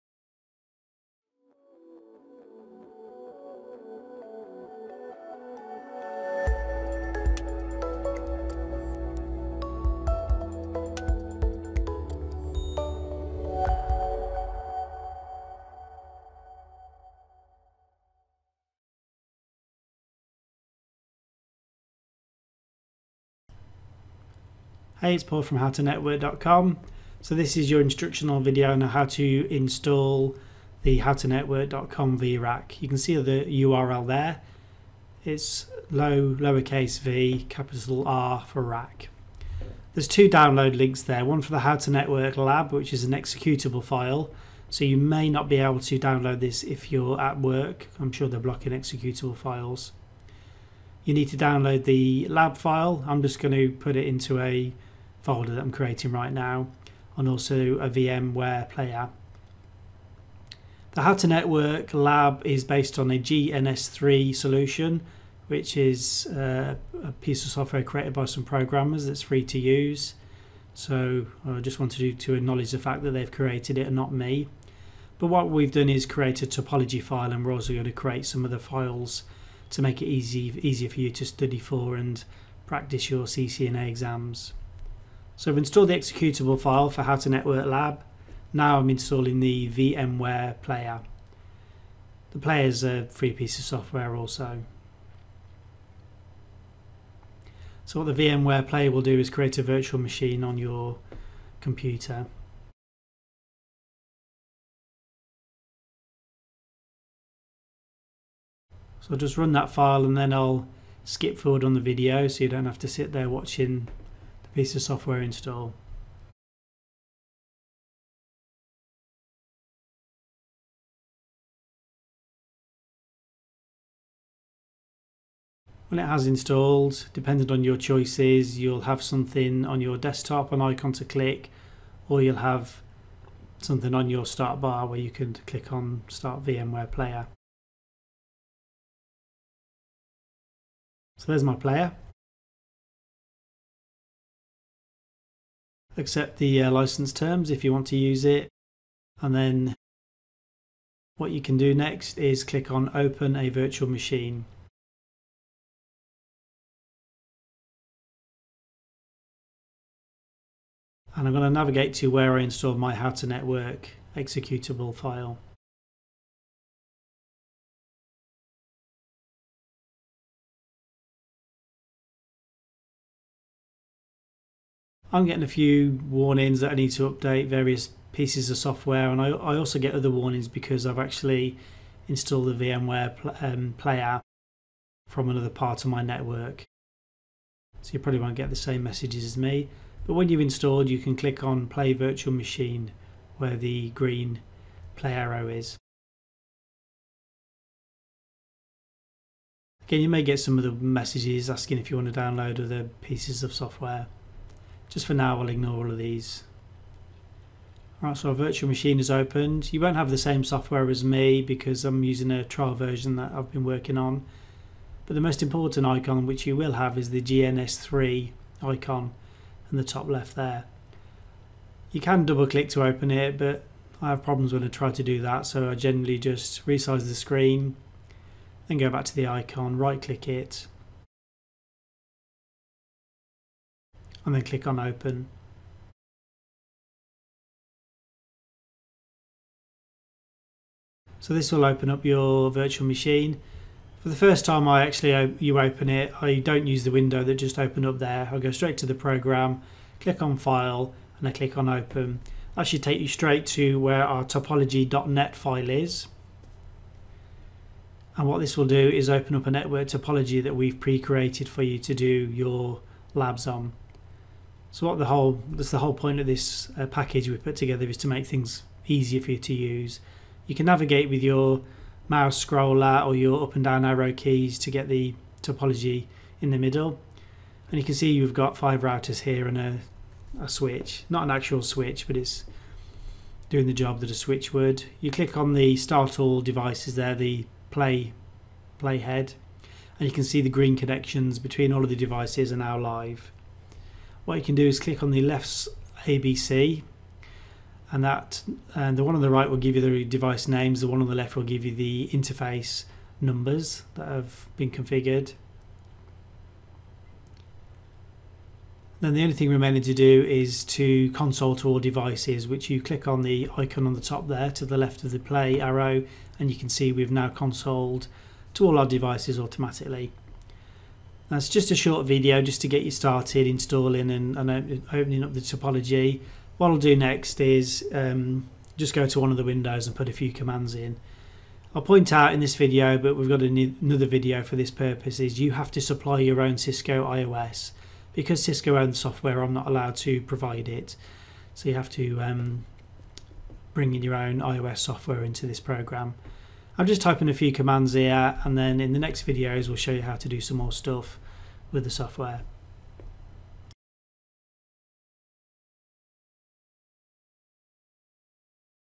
Installing Cisco vRack
- Free GNS3 based Cisco emulator. Free CCNA and CCNP labs to use for exam prep.